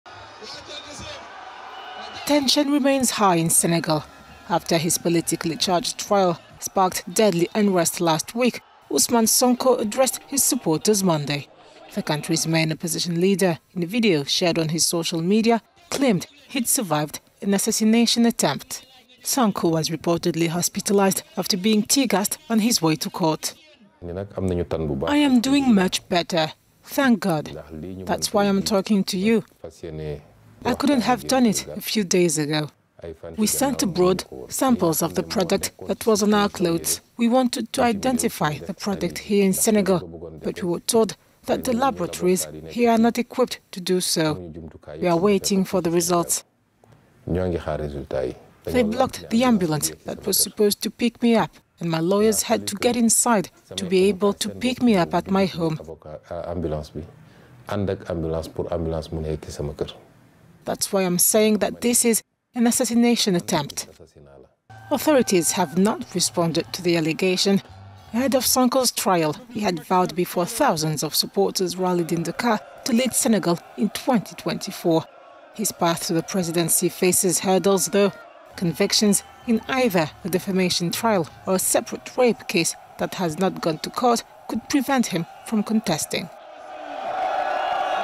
Senegalese opposition leader claims he was victim of assassination attempt
Ousmane Sonko has been receiving medical treatment since he was allegedly sprayed with an unknown "toxic product" and manhandled while authorities escorted him to a courthouse the 16th of March.